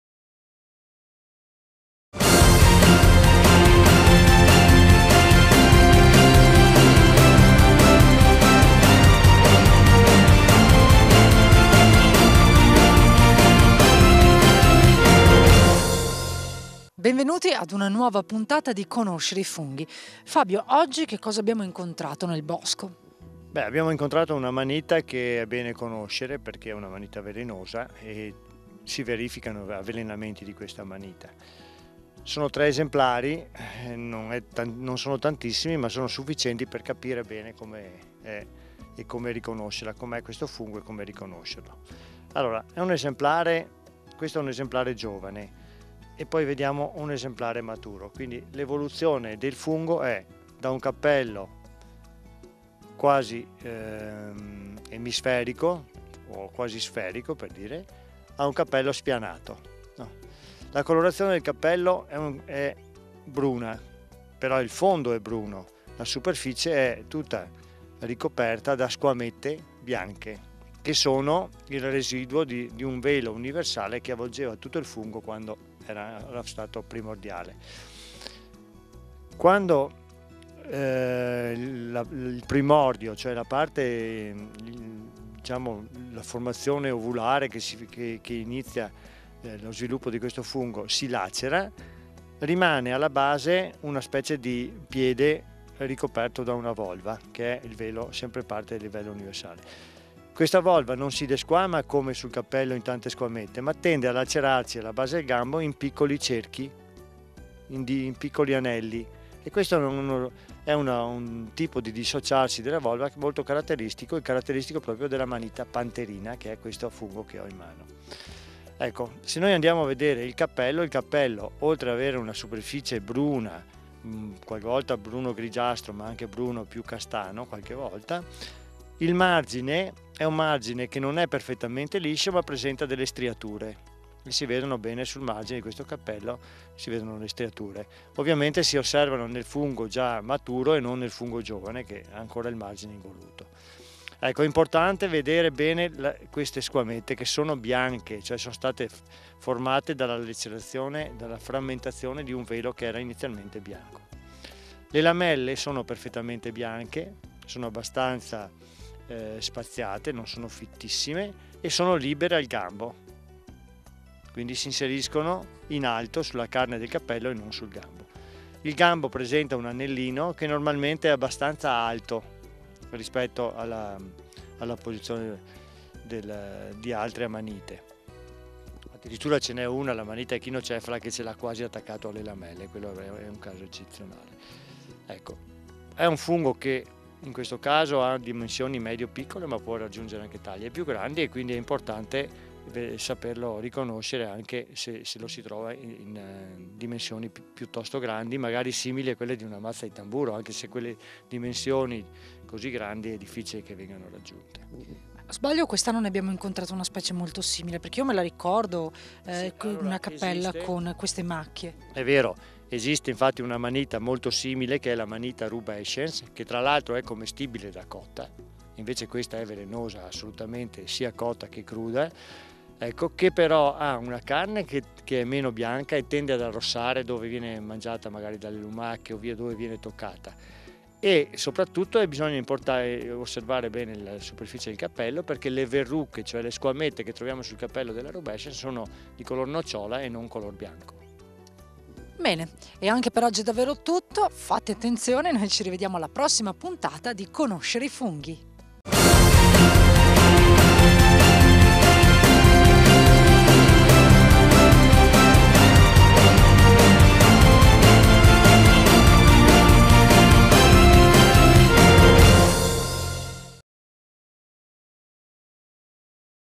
Amanita pantherina @ Conoscere i funghi 13.10.2015
Edizione 2015 di "Conoscere i funghi", un viaggio attraverso le numerose specie presenti nella provincia di Belluno e descritte da Fabio Padovan, micologo e Presidente dell'Associazione Micologica Bresadola di Belluno. L'associazione è a vostra disposizione nella nuova sede di Castion!